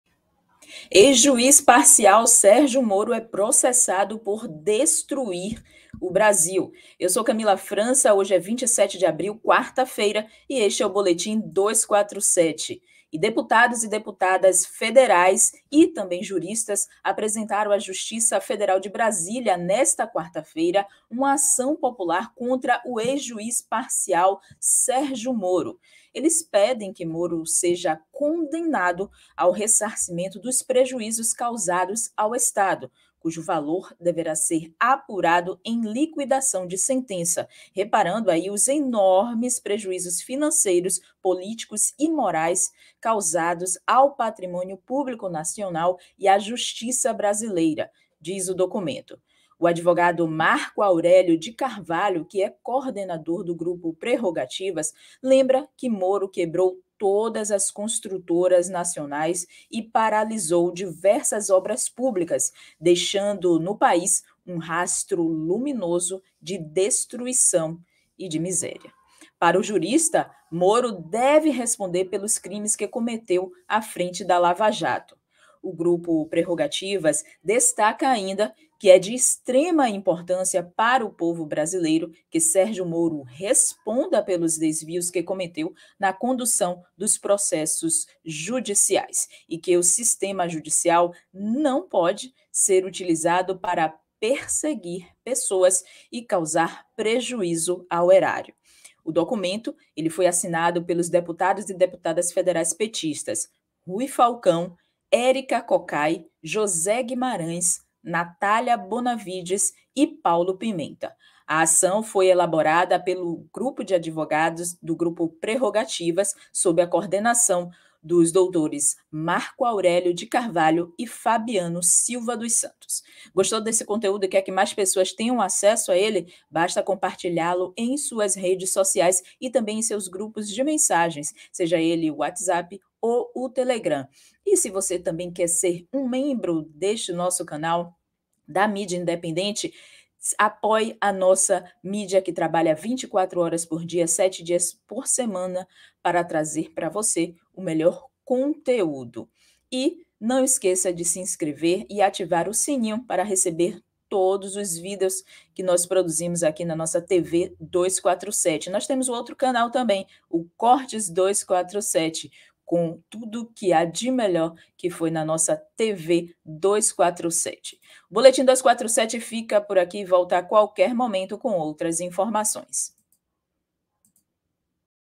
Boletim 247: Moro é processado por destruir o Brasil
A jornalista Camila França fala sobre Ação Popular apresentada por deputadas federais do PT e juristas contra o ex-juiz parcial Sergio Moro pela destruição da economia brasileira.

Você também pode fazer doação por Pix com a chave ou fazer uma assinatura Pix e pode pedir sua nota fiscal em .

Receba o boletim diário da UFMG sobre a covid no Brasil e no mundo